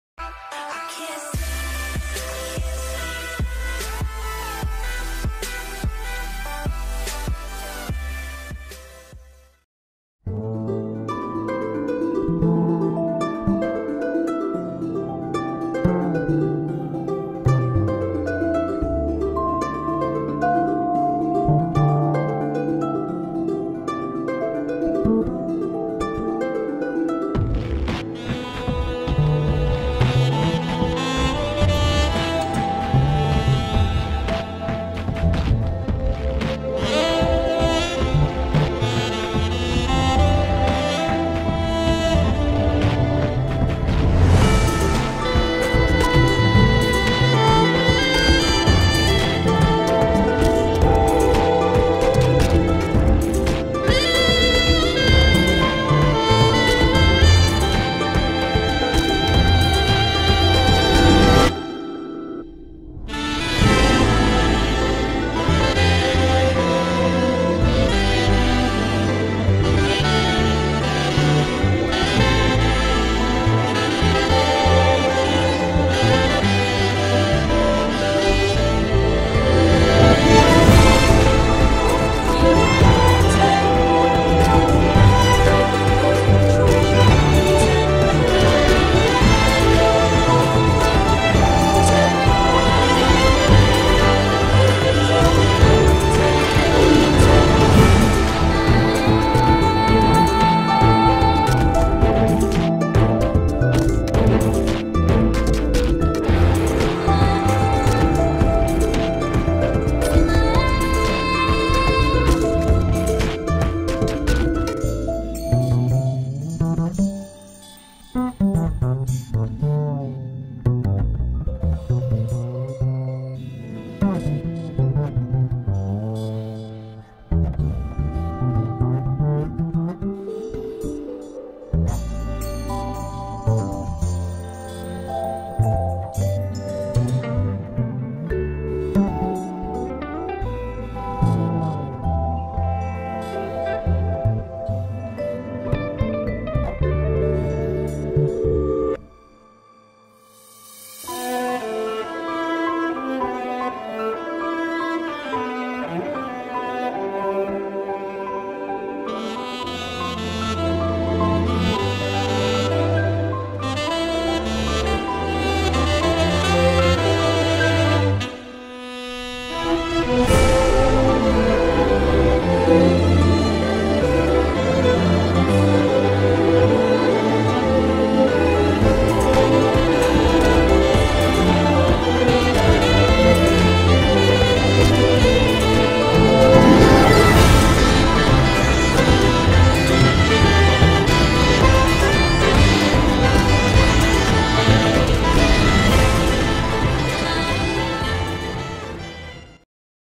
How to Draw Decim (Death Parade)
Here's Sunday's sketch video! Death Parade is one of my favorite anime's so i was happy to draw this! I'll definitely draw more Death Parade in the future!